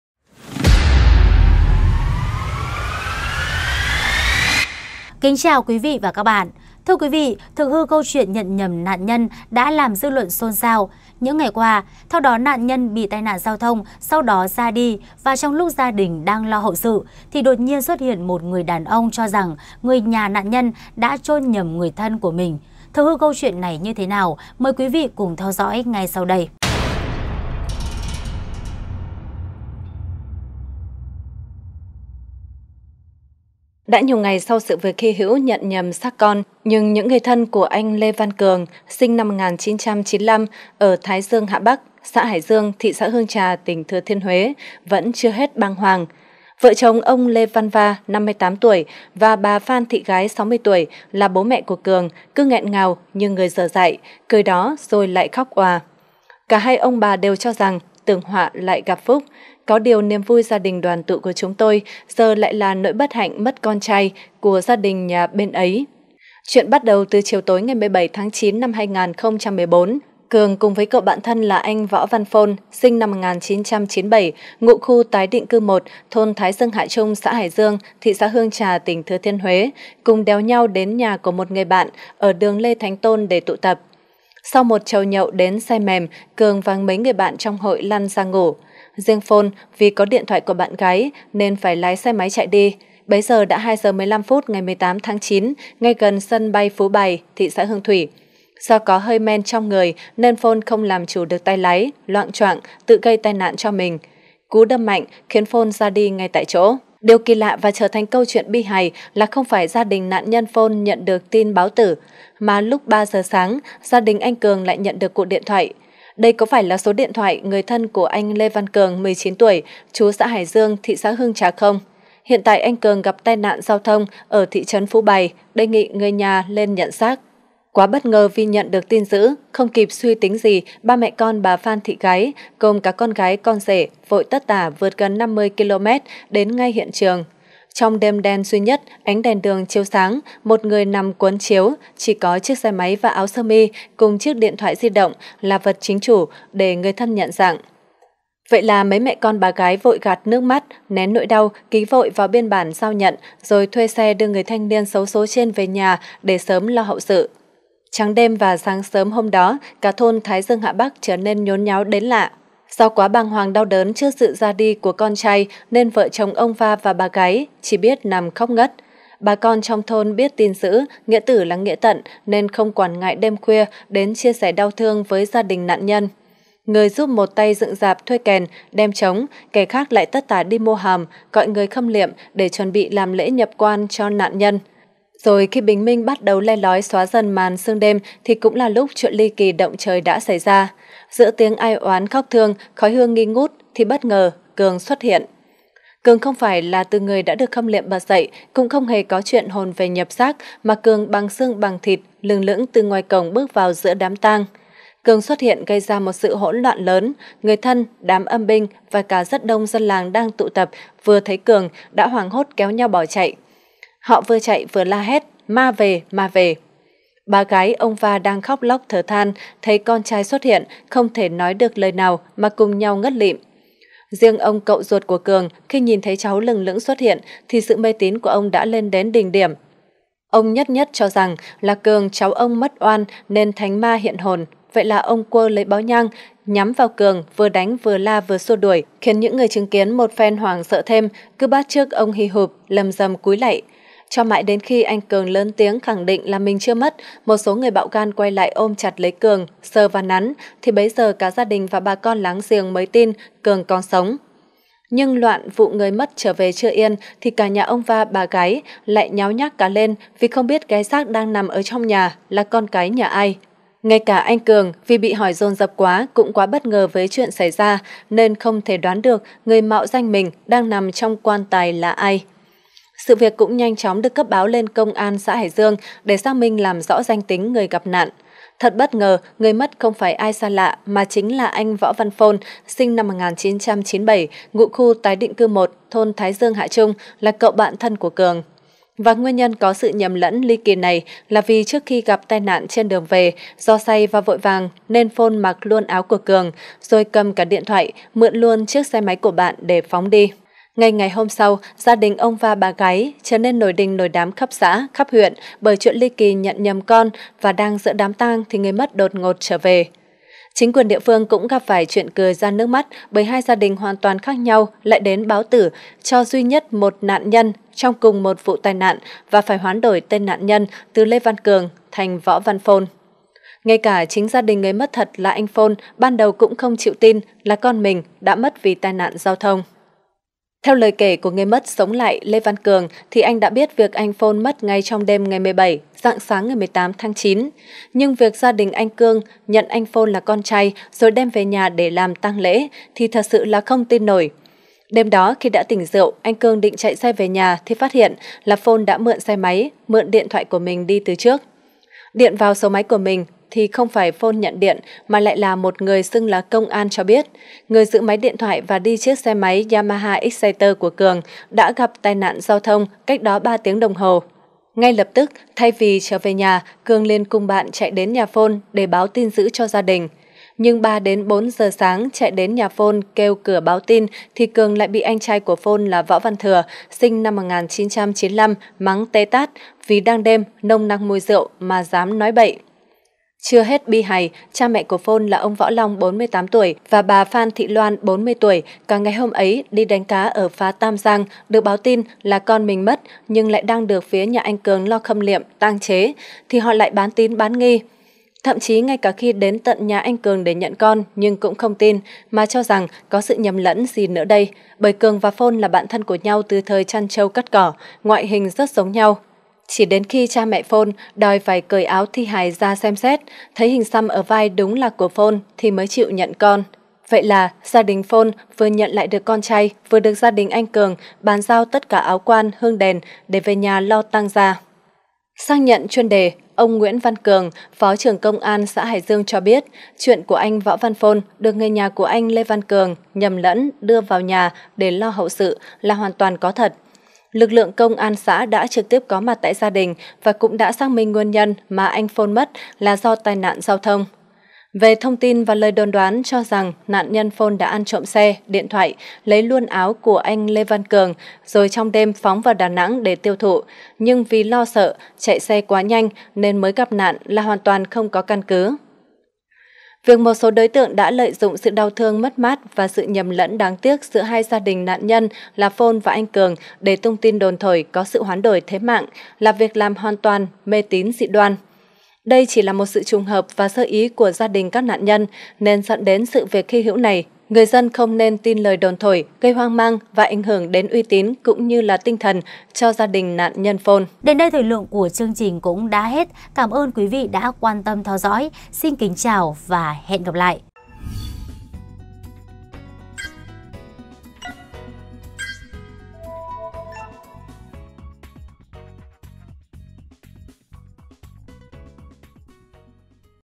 Trống kèn đang inh ỏi, người đã mất đột nhiên ngồi dậy khiến mọi người chạy tán loạn "ma về, ma về"
24H NEWS | Trống kèn đang inh ỏi, người đã mất đột nhiên ngồi dậy khiến mọi người chạy tán loạn "ma về, ma về"